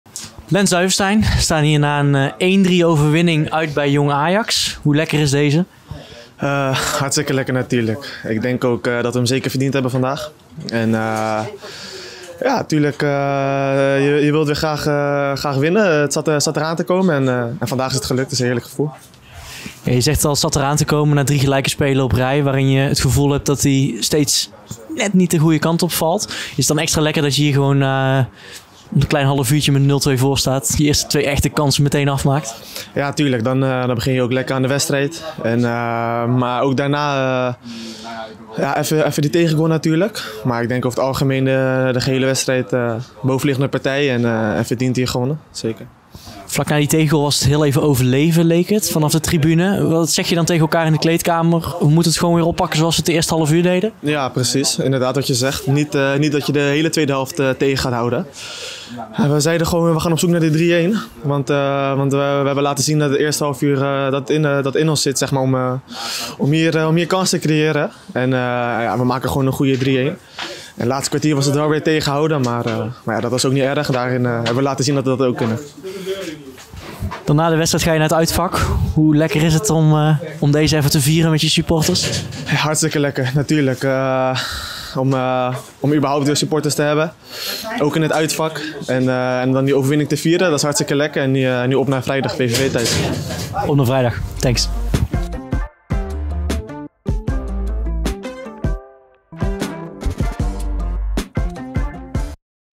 🎙 Lance Duijvestijn: 'Deze is dik verdiend!'
Lance Duijvestijn moest de wedstrijd tegen FC Emmen aan zich voorbij laten gaan vanwege een schorsing. Tegen Jong Ajax keerde de middenvelder terug in het elftal en was hij meteen belangrijk met twee assists.